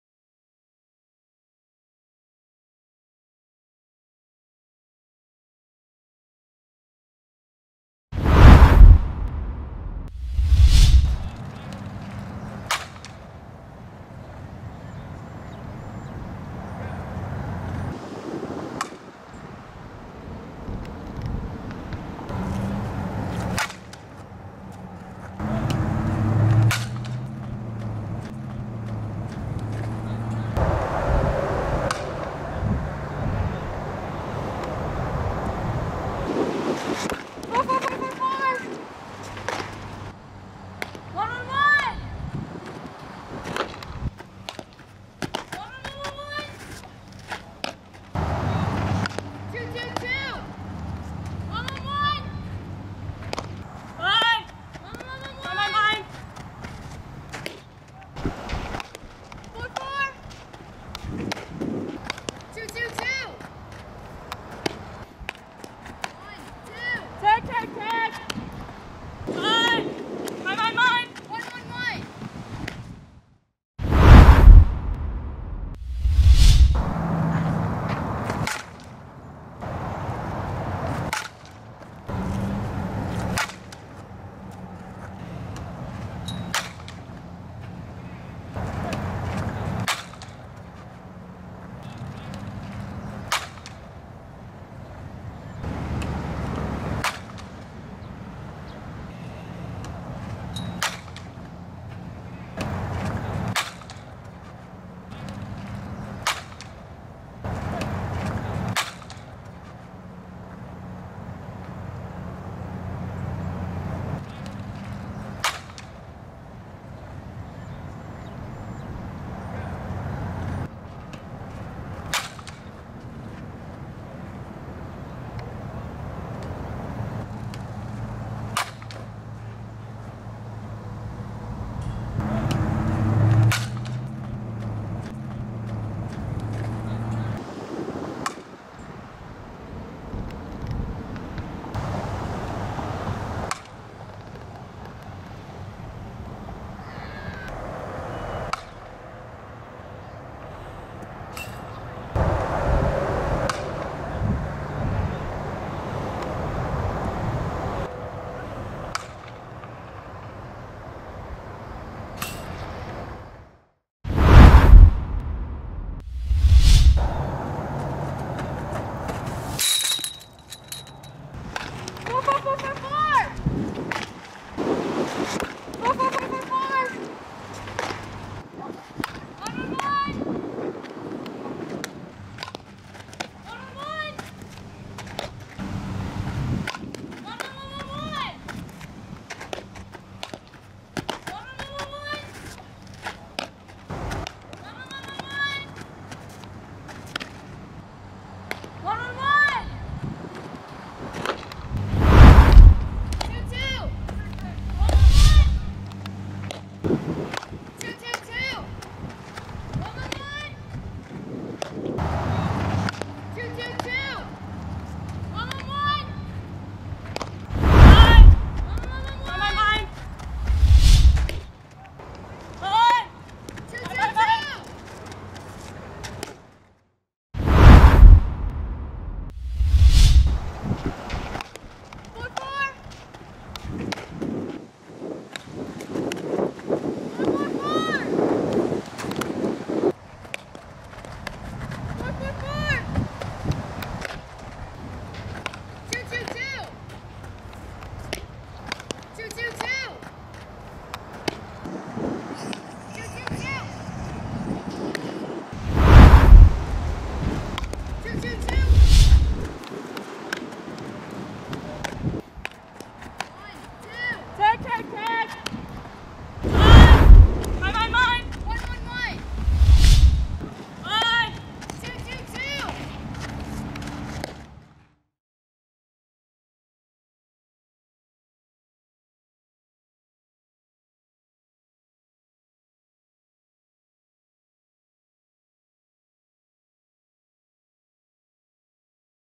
Alli Novick 2023 Corner/Power Hitter
Alli Novick 2023 Softball Skills Video Fall 2021